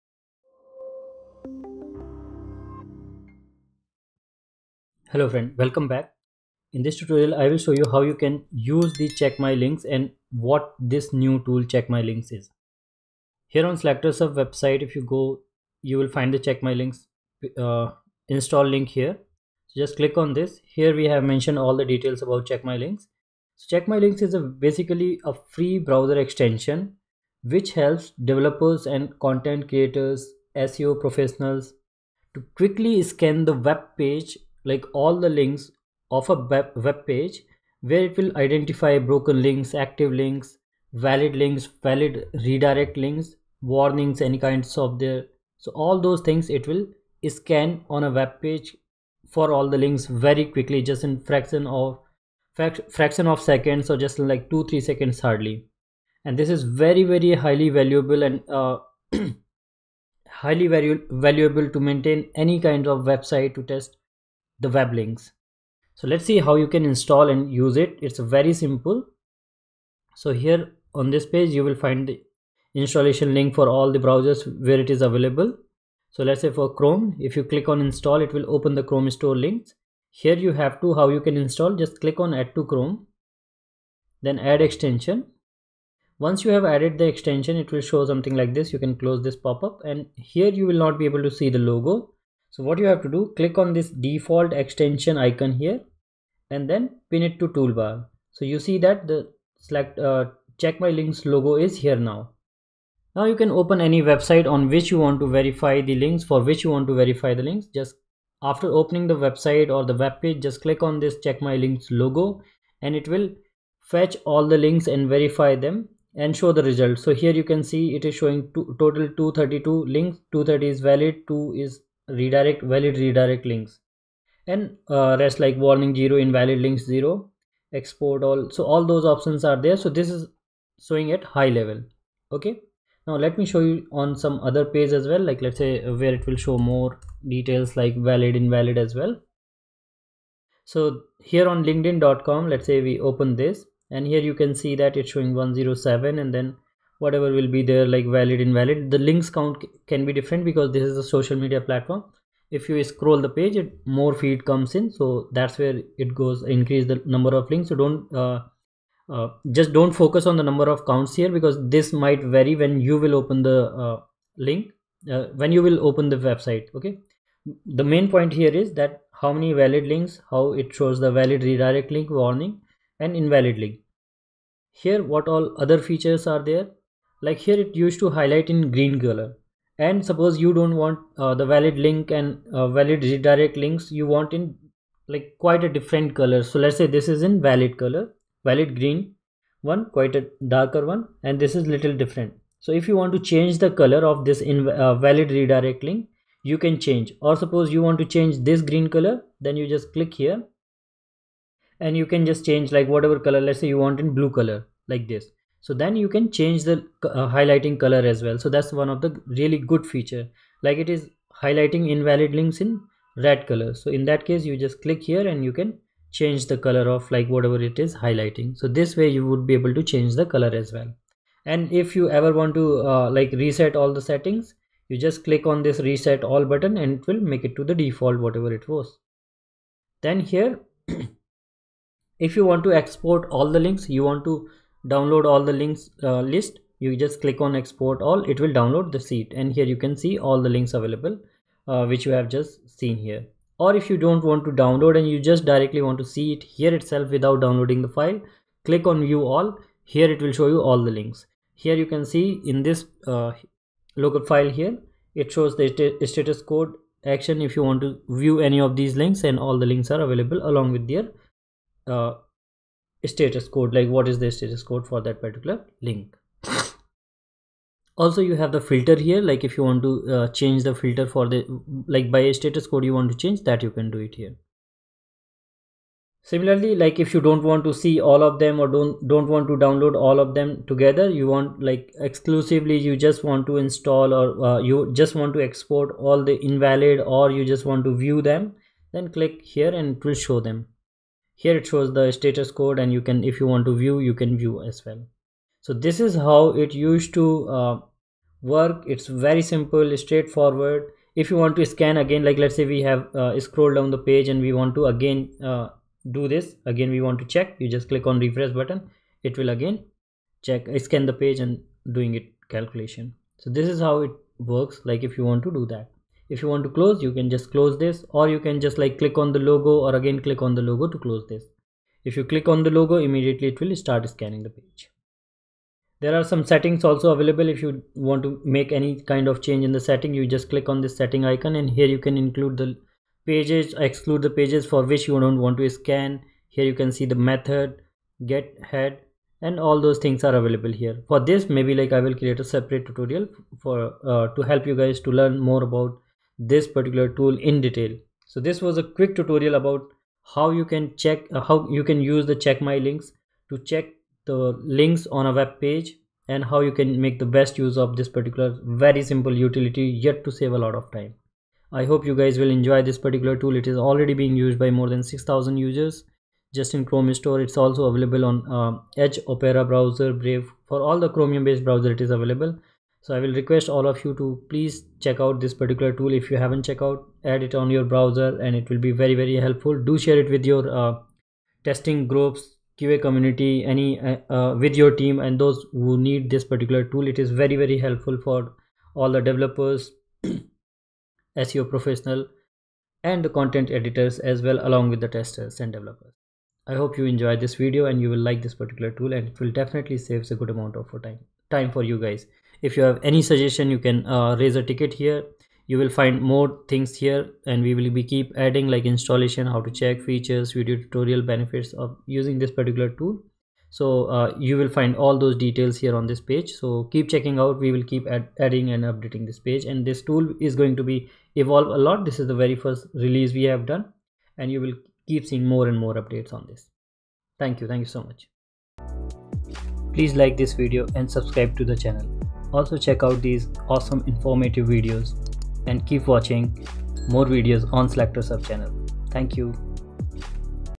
How to use Check My Links Extension | Free Browser Plugin to find Broken Links of Website in 1 Click
Check My Links is a Free Chrome extension designed to help web developers, content editors, and SEO professionals quickly scan web pages for broken links, Active links, warnings, and valid indirect links. This tool is highly valuable for maintaining website quality, improving user experience, and ensuring search engine optimization. When you’re editing a web page that has lots of links, wouldn’t it be handy to be able to quickly check that all the links on the page are working ok? That’s where ‘Check My Links’ comes in.